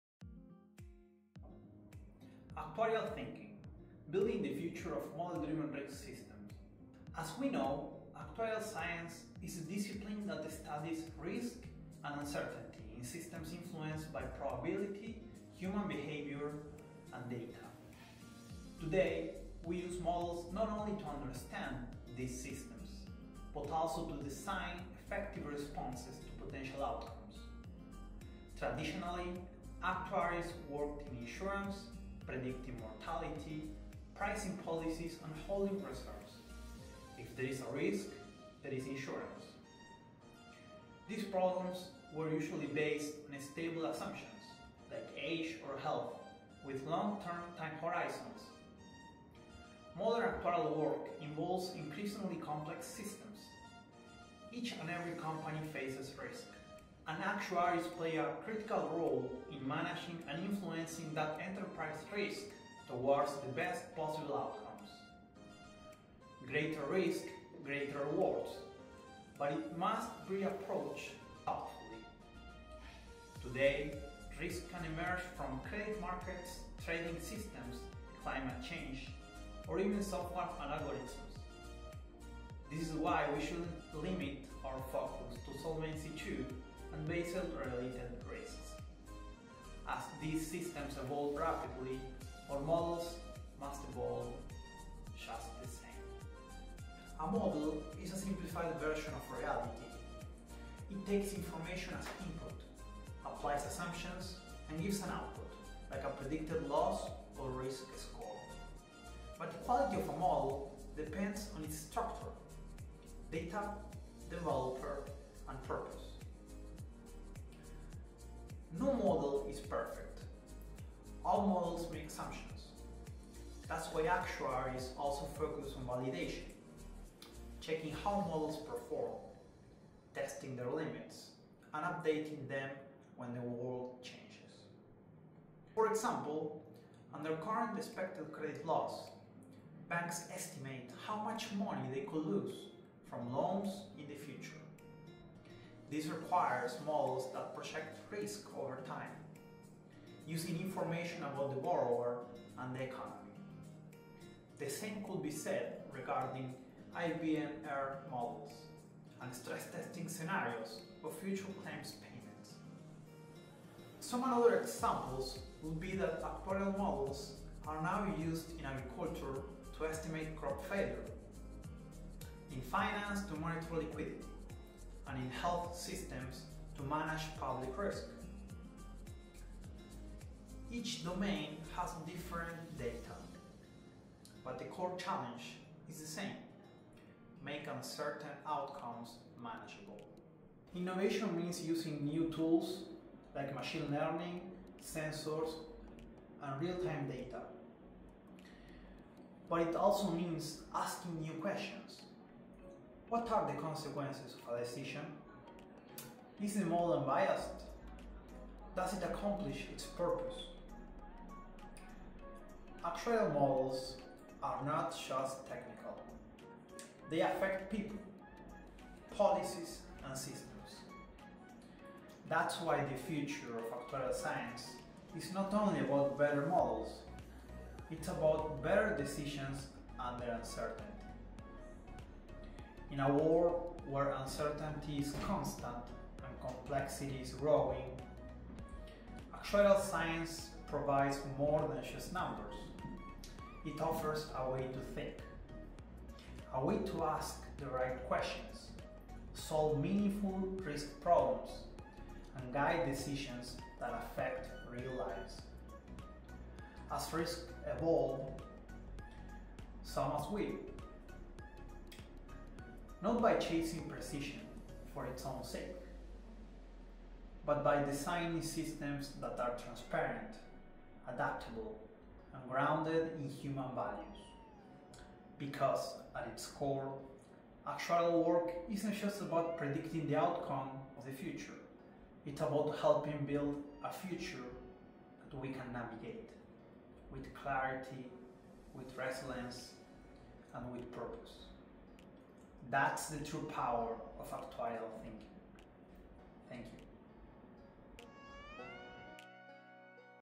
AT: Building the Future of Model Driven Risk
“Actuarial Thinking: Building the Future of Model-Driven Risk Systems” is a visual and narrative presentation into how actuarial science is evolving to meet the challenges of a data-driven, risk-intensive world. Starting from its historical roots in life insurance and mortality tables, the video explores how actuaries today engage with complex systems across industries: from finance and agriculture to public health and technology.

Through the lens of modeling, viewers discover how actuaries use structured assumptions and data to anticipate outcomes, validate model performance, and design responses under uncertainty. The film highlights the shift from traditional insurance frameworks to enterprise risk management, CECL loan loss projections, IBNR claims modeling, and stress testing, all while emphasizing the importance of model limitations, validation, and innovation.

At the end, the video argues that actuarial models are more than tools: they are decision-making frameworks that influence people, systems, and futures. As uncertainty grows, the true value of actuarial thinking lies not in predicting every outcome, but in building adaptive, transparent systems grounded in human values. This is a call to think bigger, model better, and lead the future of risk with purpose: lead the future, with actuarial thinking